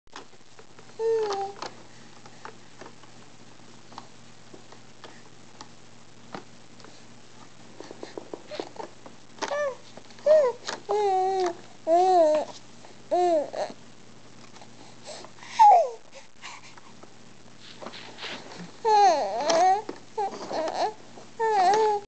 Finn in the Exersaucer
January 16th, 2007.   Finn playing in her new exersaucer